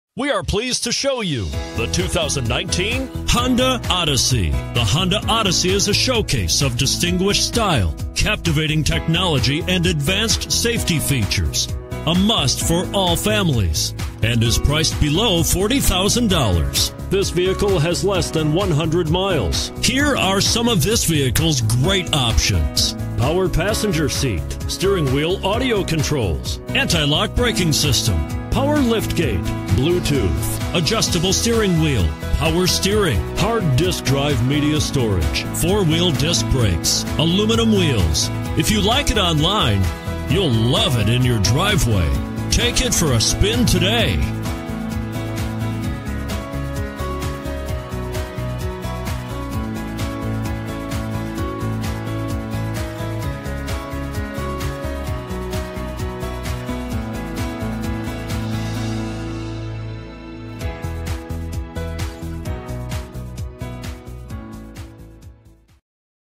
2019 Honda Odyssey HN190907
2019 HONDA ODYSSEY Nanuet, NY
Stock# HN190907

Moonroof, Heated Leather Seats, Third Row Seat, Aluminum Wheels. White Diamond Pearl exterior and MOCHA interior, EX-L trim. FUEL EFFICIENT 28 MPG Hwy/19 MPG City! Warranty 5 yrs/60k Miles - Drivetrain Warranty; AND MORE!

Leather Seats, Third Row Seat, Sunroof Honda EX-L with White Diamond Pearl exterior and MOCHA interior features a V6 Cylinder Engine with 280 HP at 6000 RPM*. Rear Spoiler, MP3 Player, Power Third Passenger Door, Remote Trunk Release, Keyless Entry, Privacy Glass, Child Safety Locks.